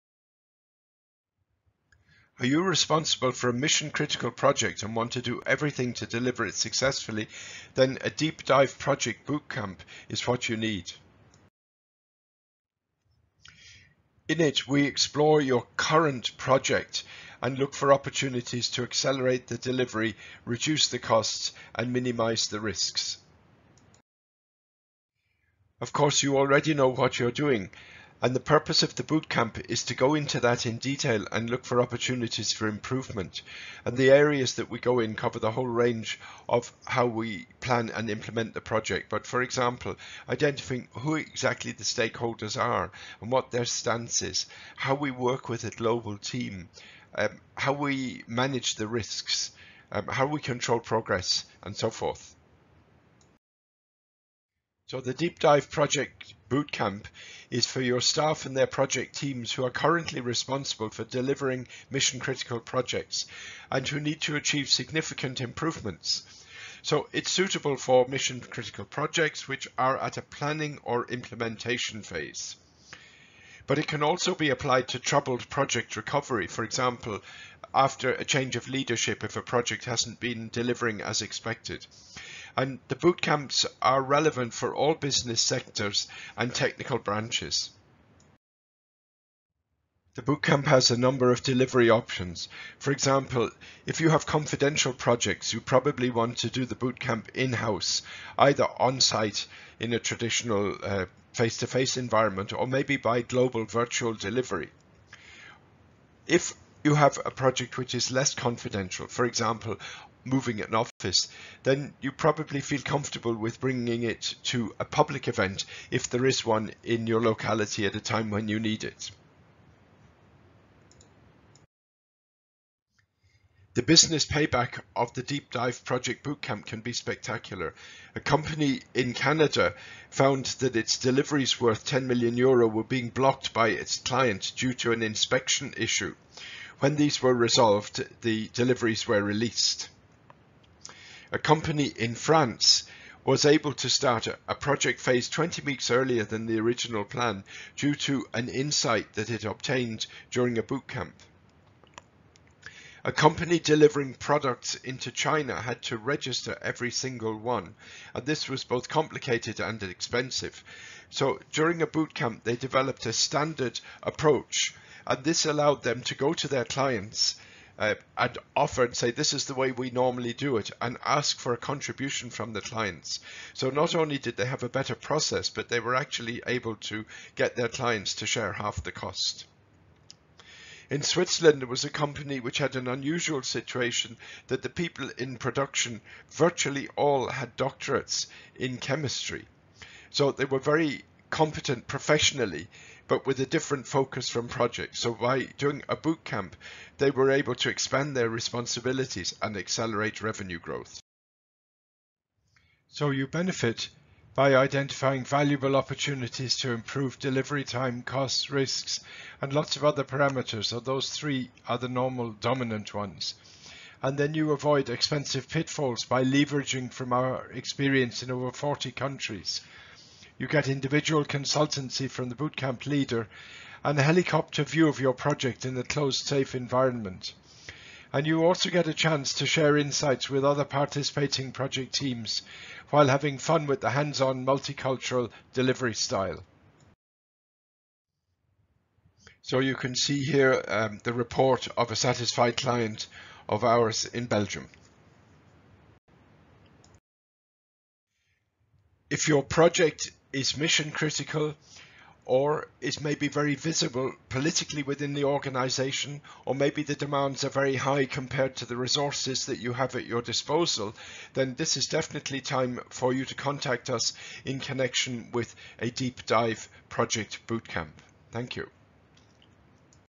DeepDive Project Bootcamp mini brochure
Who and why should you consider a DeepDive Project Bootcamp?  This short video explains the key points.

Bootcamps are suitable for current projects in the following situations:
-mission-critical
-recovery of troubled project
-very visible within the organization
-very demanding with the resources available
-risky etc. etc.

This video replaces the one entitled: "DeepDive Project Bootcamp Mini-Brochure with commentary"